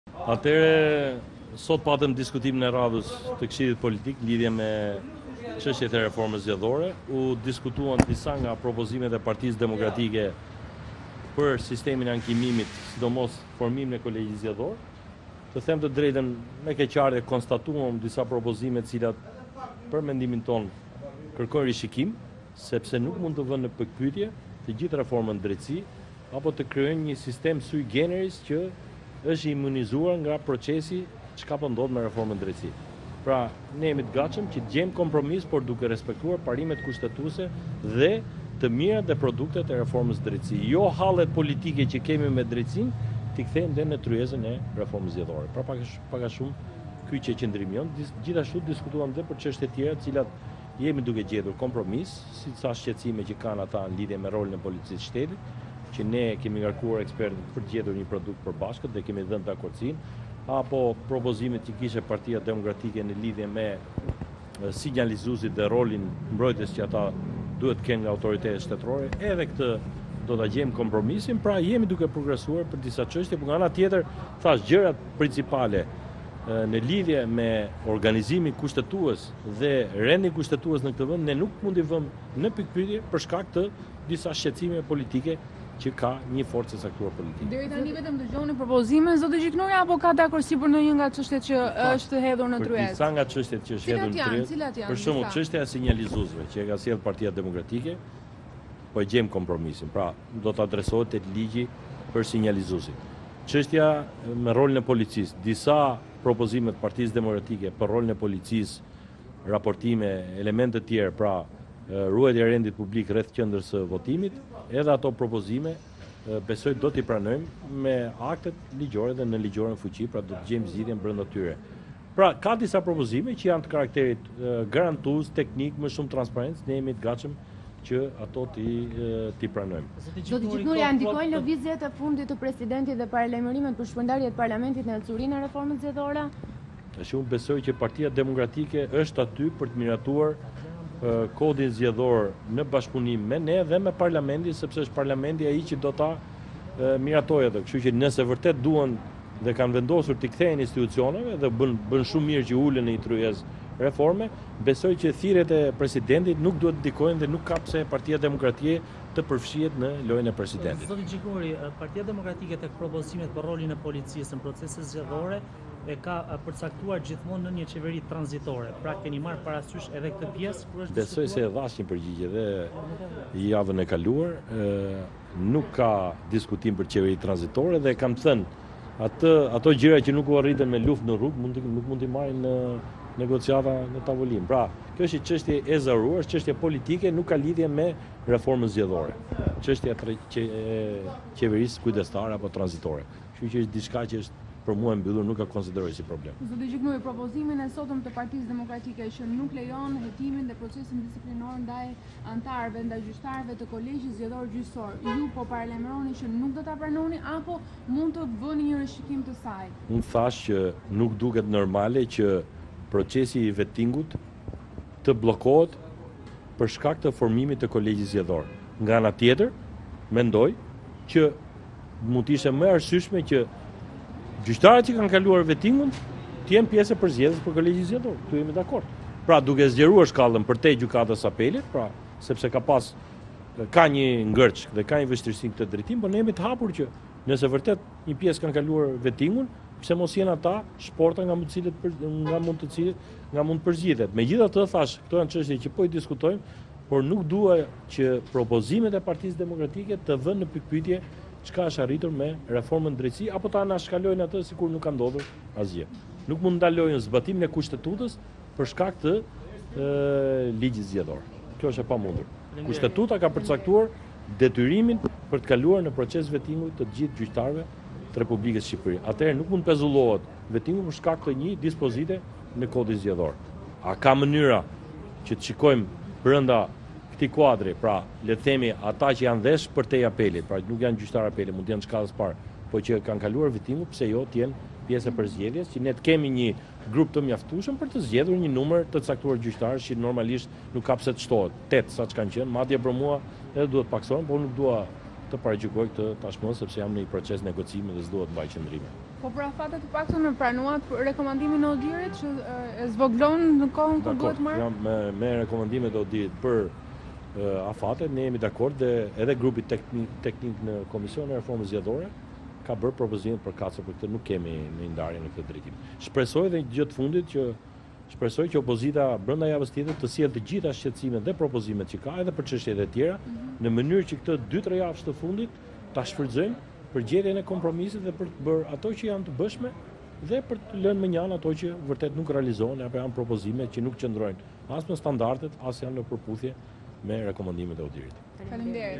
Moratoriumi për gjyqtarët e Kolegjit, PS-ja kundër: Vendosin në pikëpyetje drejtësinë e re
Shumica është kundër propozimit të opozitës që gjyqtarët e Kolegjit Zgjedhor të mos futen në vetting për 4 vite që do të jenë pjesë e institucionit që do të shyqrtojnë ankimimet për zgjedhjet. Damian Gjiknuri doli kundër dhe ngriti pyetjen se përse të mos zgjidhen ata që e kanë kaluar vettingun? Ai u shpreh i hapur për pjesën tjetër të propozimit, që disa nga anëtarët të zgjidhen nga gjykatat e shkallës së parë dhe jo vetëm të Apelit.

"Me keqardhje konstatuam disa propozime që duan rishikim, se nuk mund të vendosin në pikëpyetje të gjithë reformën në drejtësi. Ne jemi gati të gjejmë kompromis por duke respektuar parimet kushtetuese dhe jo hallet që kemi me reformën në drejtësi ti kthejmë në tryezë. Nuk duket normale që procesi i vettingut të bllokohet për shkak të formimit të kolegjit zgjedhor. Mund të ishte më e arsyeshme që gjyqtarët që kanë kaluar vettingun të jenë pjesë e procesit për Kolegjin Zgjedhor. Pse mos të jenë ata shporta nga e cila të përzgjidhet? Nuk dua që propozimet e PD-së të vendosin në pikëpyetje atë që është arritur me reformën në drejtësi. Kushtetuta ka përcaktuar detyrimin që cdo gjyqtar të kalojë në vetting dhe kjo nuk mund të pezullohet. Ata që janë përtej apelit për të plotësuar trupën pse jo mos të jenë. Me rekomandimet e ODHIR për afatet jemi dakord dhe nuk kemi ndonjë ndarje në këtë drejtim. Shpresoj që opozita këtë 2-3 javë që kanë mbetur ta shfrytëzojmë për kompromis dhe të lëmë mënjanë ata që nuk janë në përputhje me rekomandimet e ODHIR".